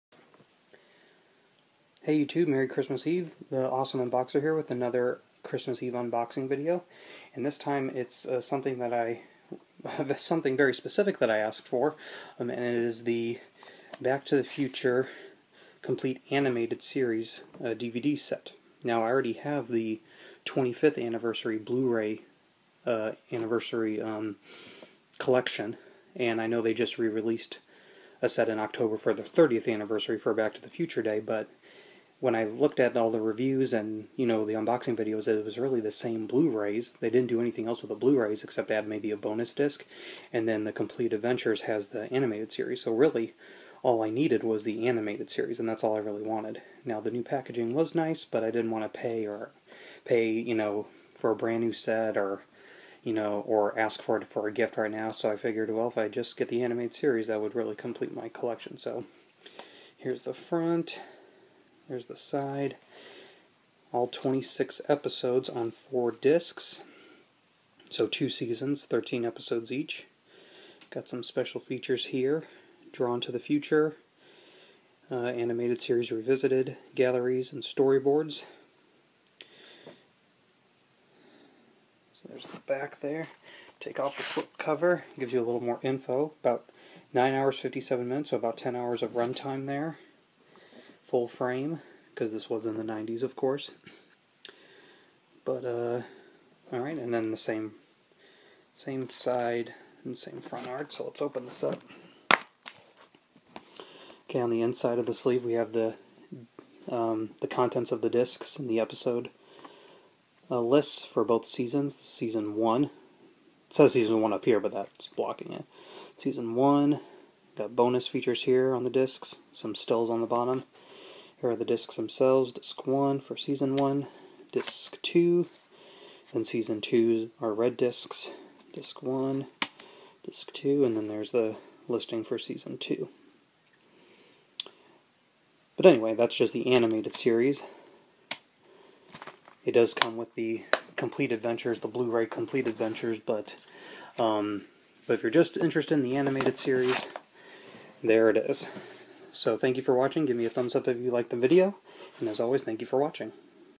Back to the Future Complete Animated Series (DVD) unboxing - Christmas Eve 2015
Unboxing of the Back to the Future Complete Animated Series DVD.